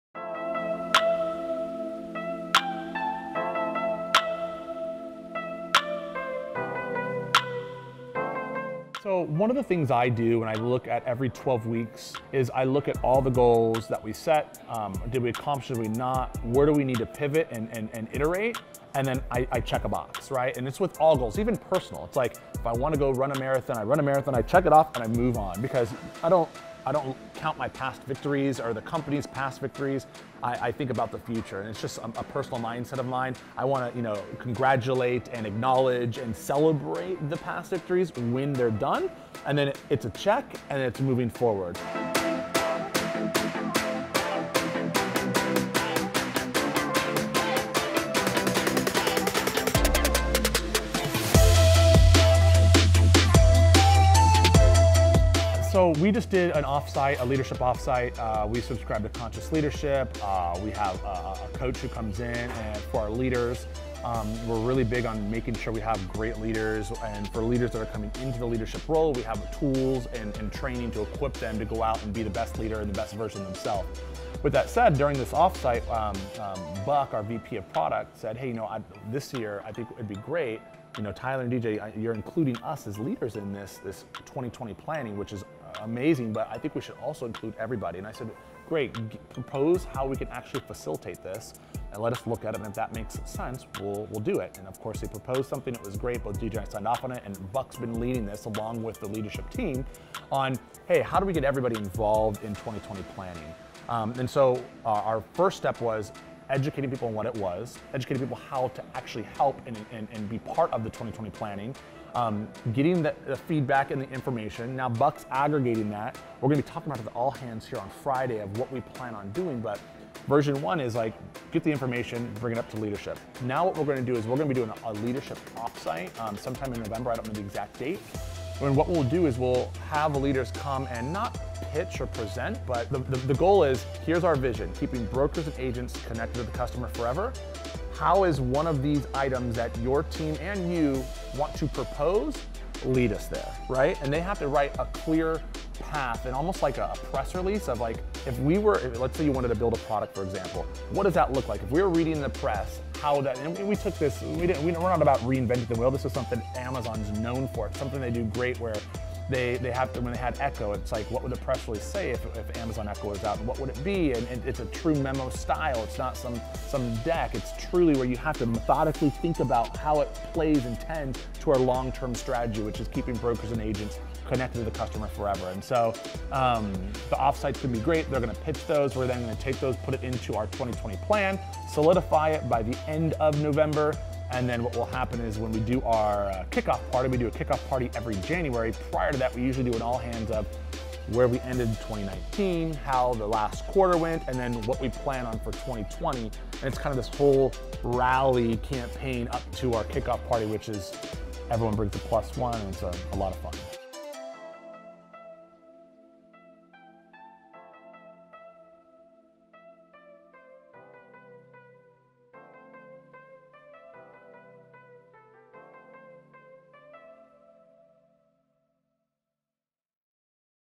TVP29 - 2020 Vision
We decided to shake things up recently, by trying something new with how we approach big goals for the coming year. We had recently brought in all the department managers to help us set goals, but this time we heard a suggestion to step it up a notch and include the entire company in this decision-making process. So we mapped it out, launched it, and reviewed the results. Guess what? It was a hit. We empowered our employees to have a stake in what we pursue as a company, as a family, and they hit the ground running with it. The year 2020 is all about vision(pun definitely intended), and we’re excited to have every single hand on deck to make sure it’s the best year ever… hit play and get inspired.